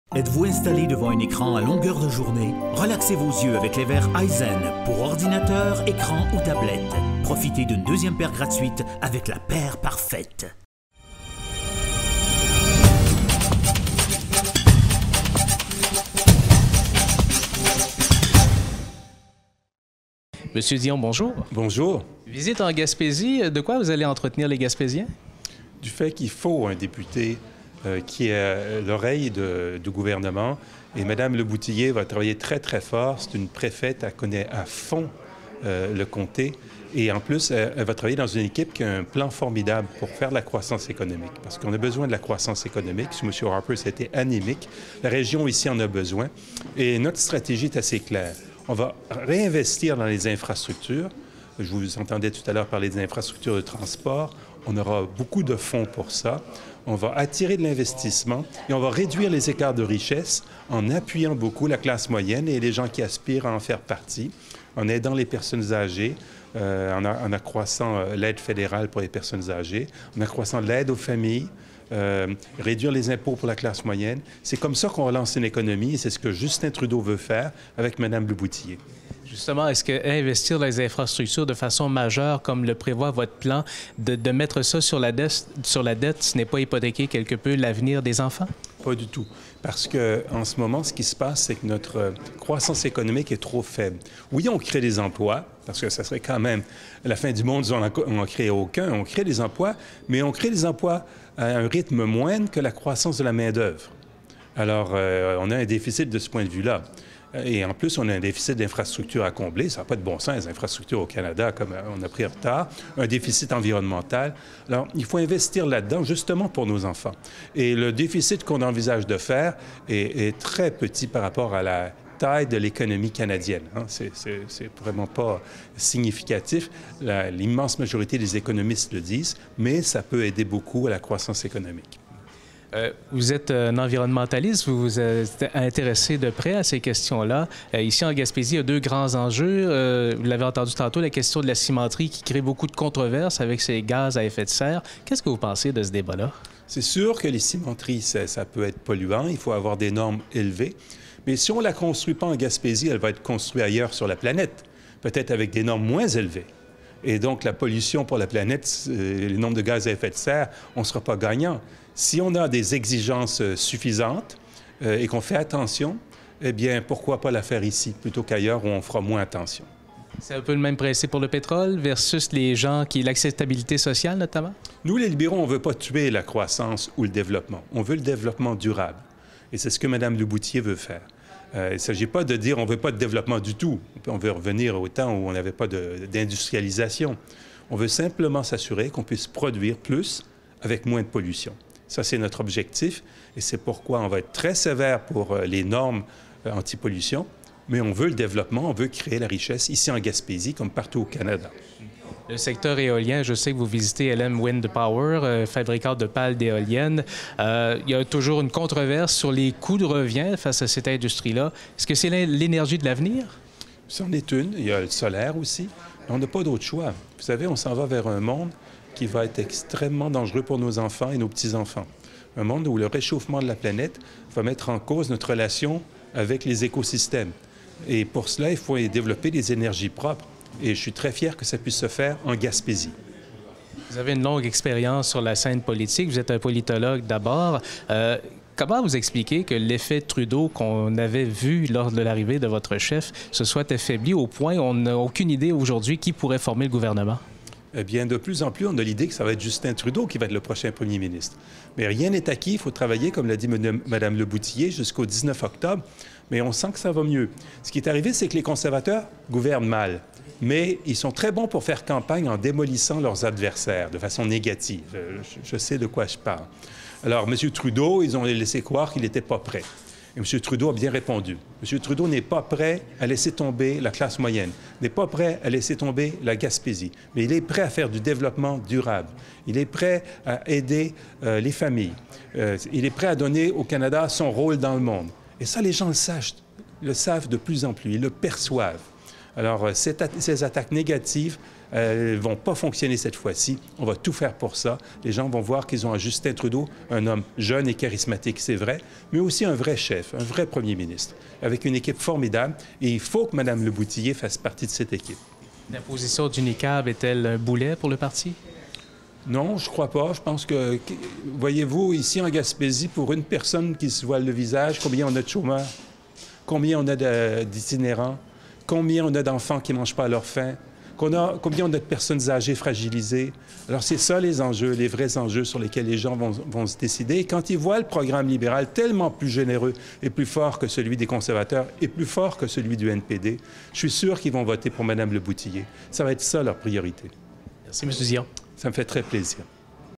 Stéphane Dion Web
Stéphane Dion est venu donner un coup de main à Diane Lebouthillier dans le cadre de la campagne électorale. Nelson Sergerie l'a rencontré.